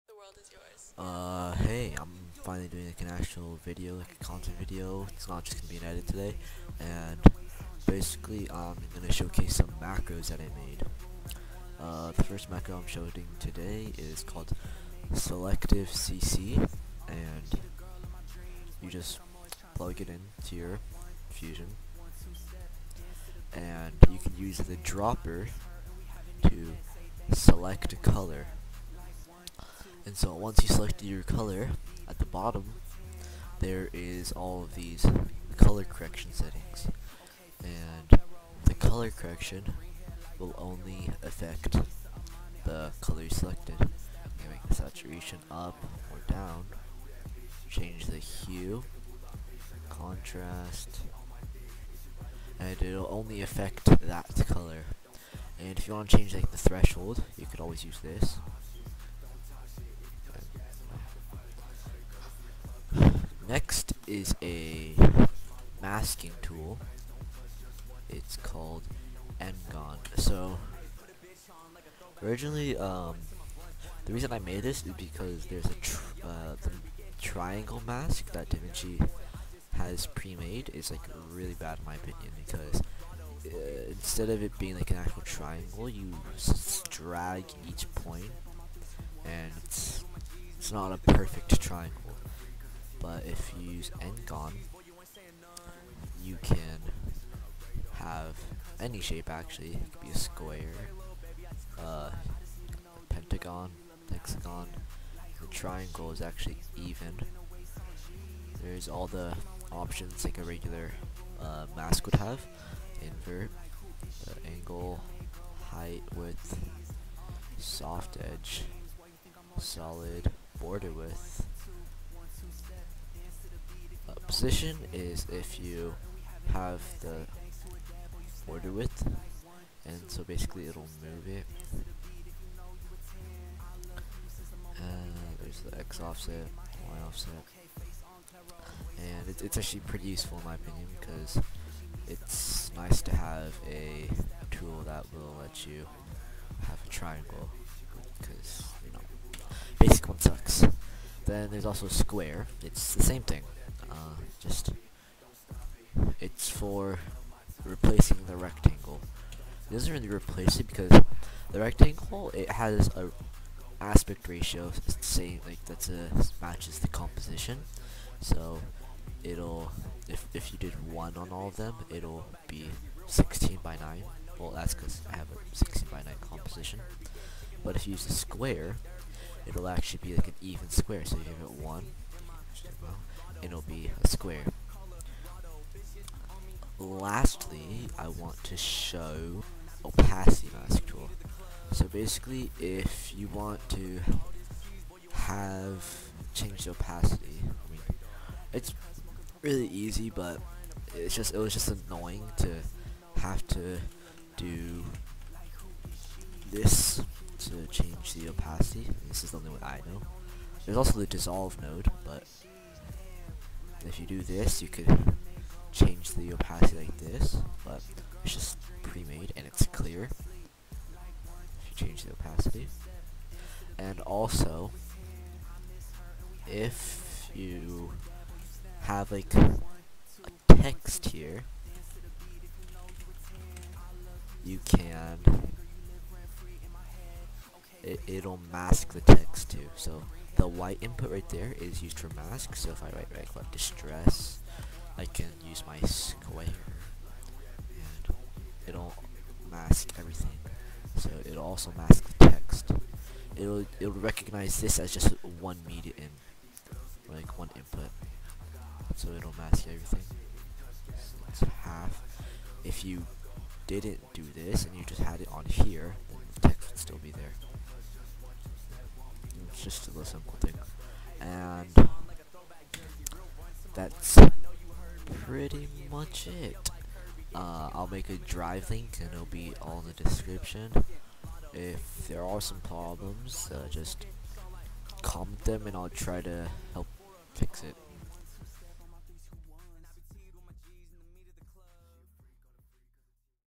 Color Isolation Macro + More On DaVinci Resolve ( Free Effects )
In this video, I show 4 macros I made while just messing around in DaVinci.
I plan to make some type of tutorial next and wonder if you guys wanted a tutorial on the Neeqs transition. Like the tutorial Joshyy did but for DaVinci.